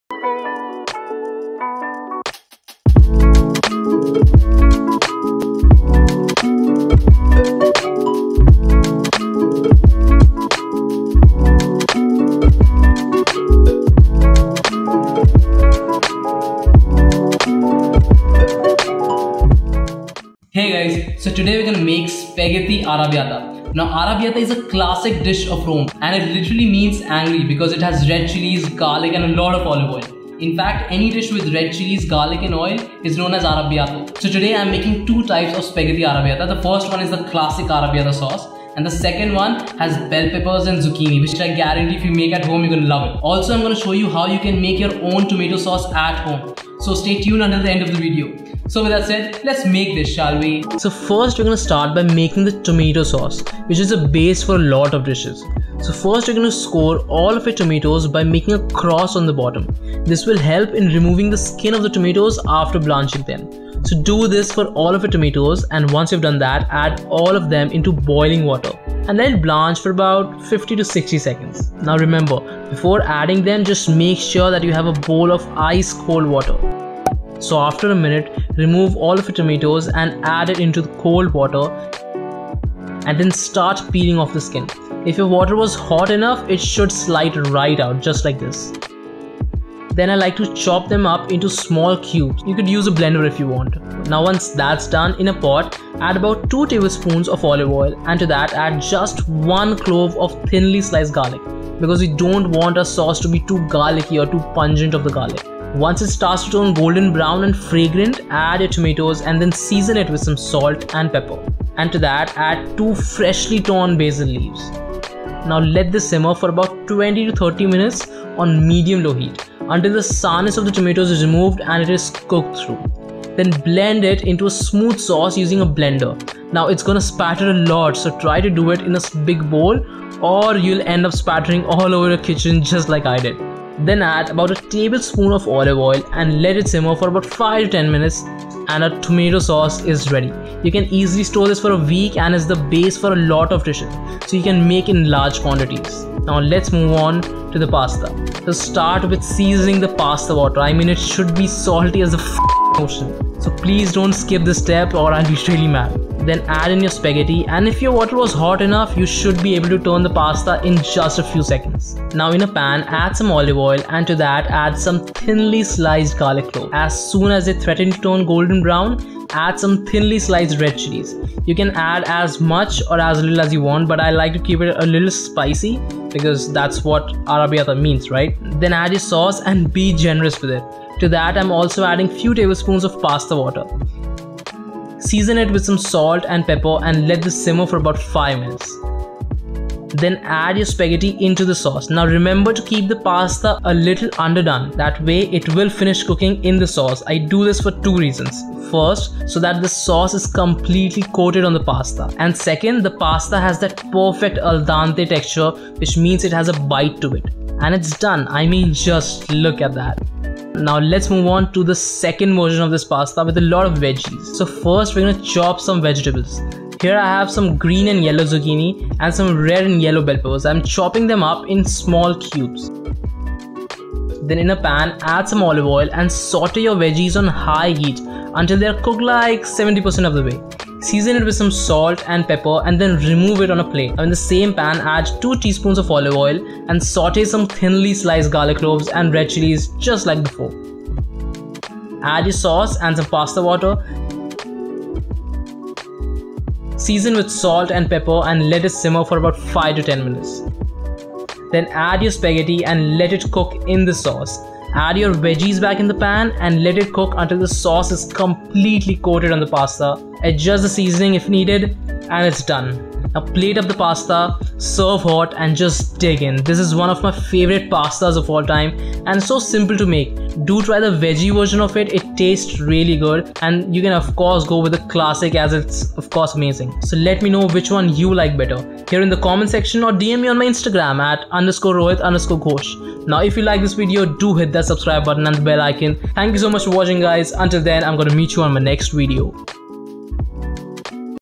SPAGHETTI ARRABBIATA | How to make a Classic Arrabbiata Sauce
INGREDIENTS

TOMATO SAUCE-

1 Kg Onion
1-2 cloves of Garlic
3-4 tbsp Olive oil
2-3 nos. Basil Leaves

CLASSIC SPAGHETTI ARRABBIATA

1 tbsp Olive oil
 2 nos. Garlic Cloves
2-3 nos. Red Chillies
1 cup Tomato Sauce
80 gm Spaghetti
2-3 tbsp Pasta Water
Salt & pepper to taste
Grated Parmesan for Garnish (optional)

SPAGHETTI ARRABBIATA  2.0 (with Zucchini and Bell Peppers)

4 tbsp Olive Oil
1 nos. Zucchini (Green and Yellow)
1 nos. Bell pepper (Red and Yellow)
2 nos. Garlic Cloves
2-3 nos. Red Chillies
1 cup Tomato Sauce
80 gm Spaghetti
2-3 tbsp Pasta Water